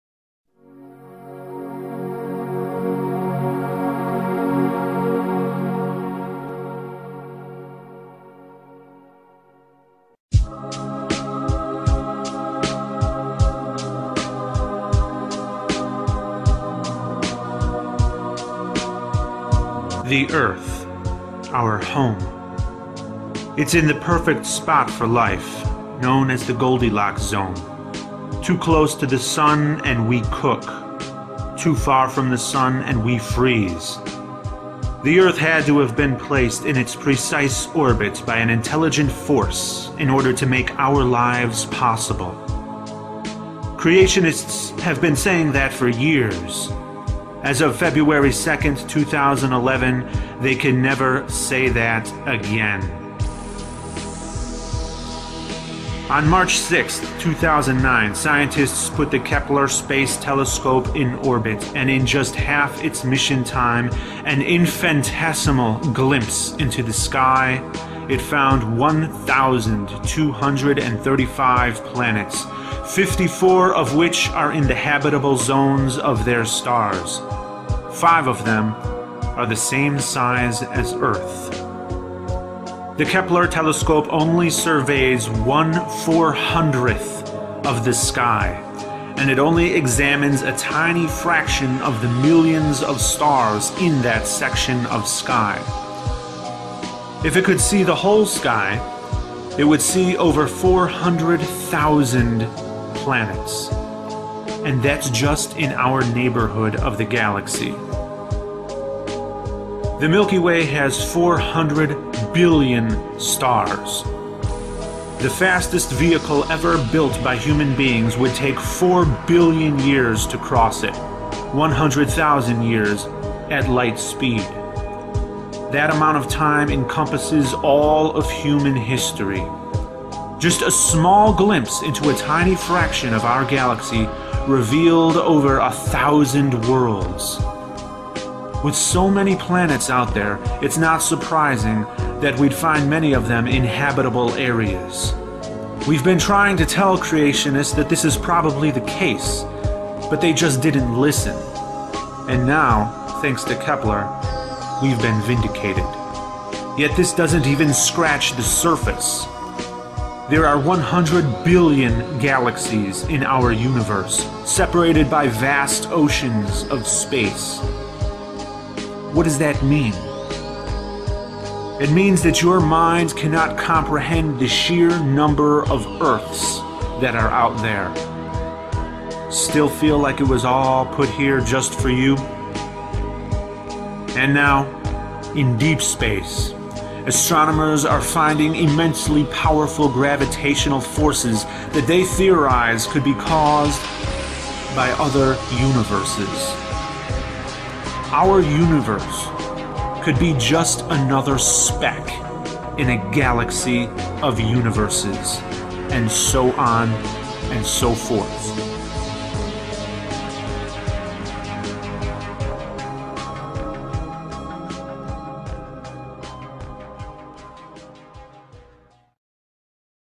Yaratılışçılar, Yine Kaybetti..! Bilim, bu yüzden bu kadar baskı altında...!
6 Mart 2009'da KEPLER UZAY TELESKOBU Dünya yörüngesine yerleştirildi ve daha görev süresinin yarısında gezegene ufak bi bakış ile 1235 gezegen buldu...
Bunların 54 tanesi yıldızlarına hayatın mümkün olabileceği ideal uzaklıktalar...
5'i Dünya ile aynı büyüklükte. Kepler teleskobu daha gökyüzünün sadece 1/400'ünü inceledi... Eğer gökyüzünün tamamını inceleyebilseydi... 400,000+ gezegenden daha da fazla bulurdu... Ve bunlar sadece galakside bizim bulunduğumuz civarda. Samanyolu'nda 400 milyar yıldız var.